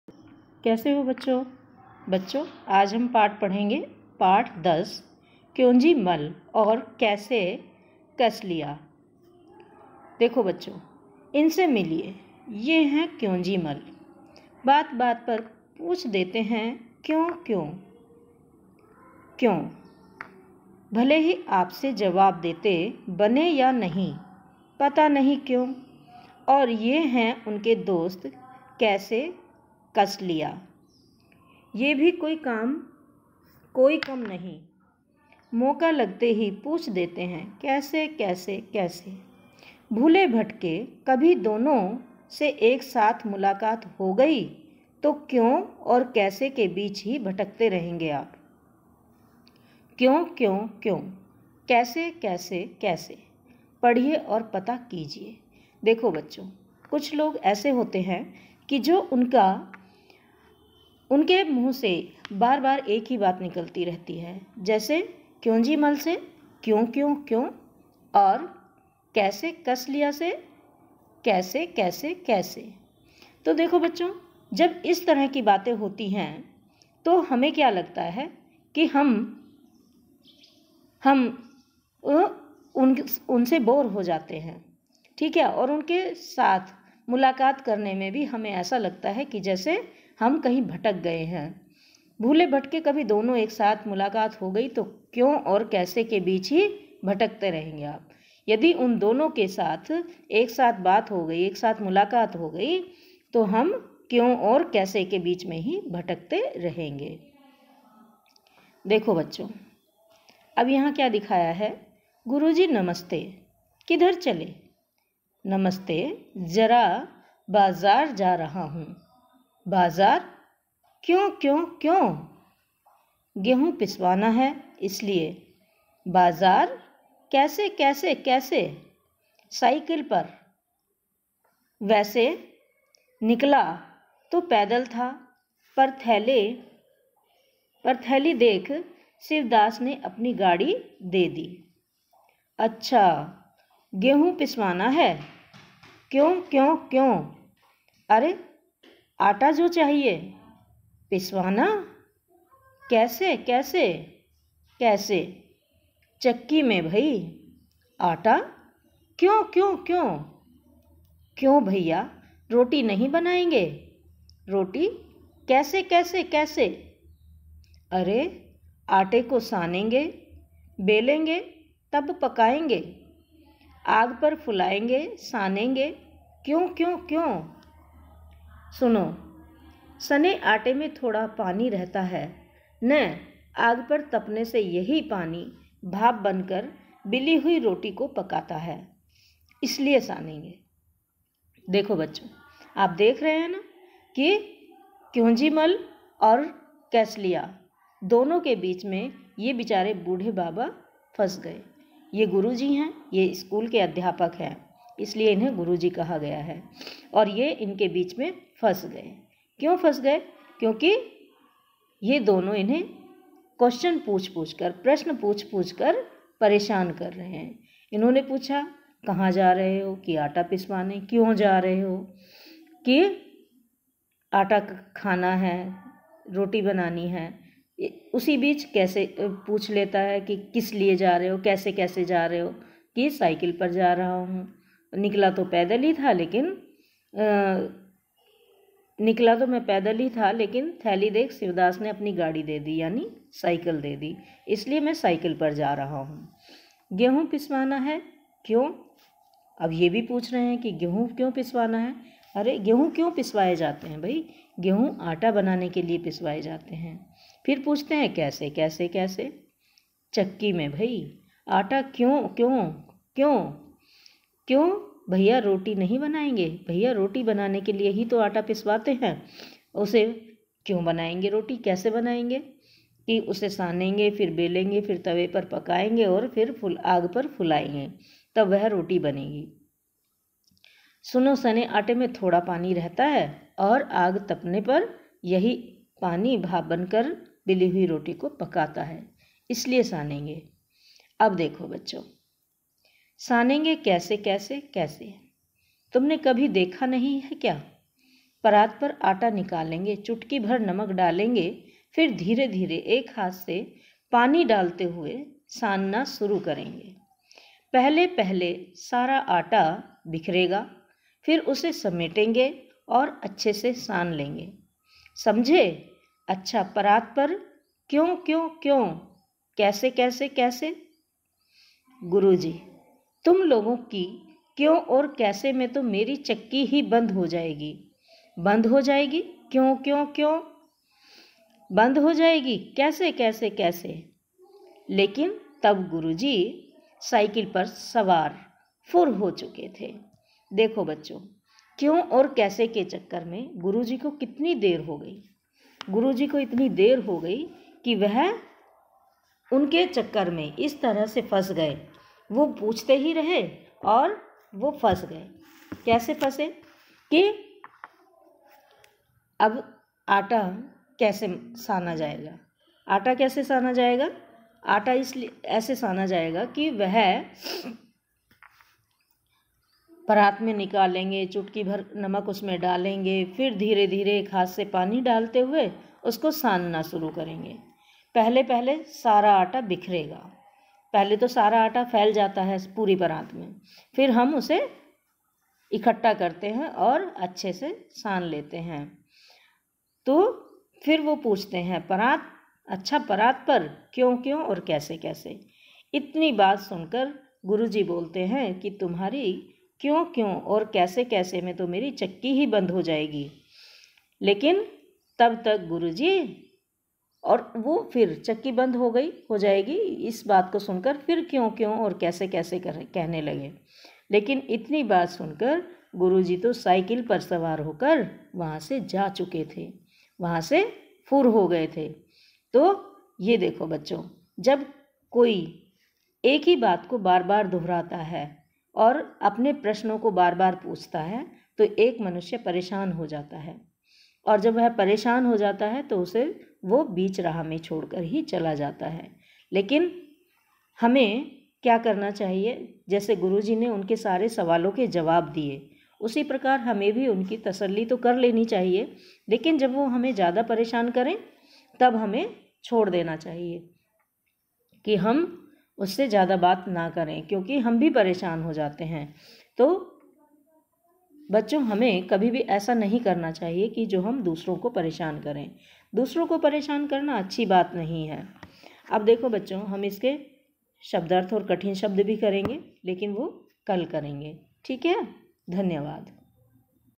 28 अक्टूबर कक्षा तीन विषय हिंदी पाठ 10 क्यों जीमल और कैसे कैसे लिया का स्पष्टीकरण कराया
पाठ 10 का पाटन करें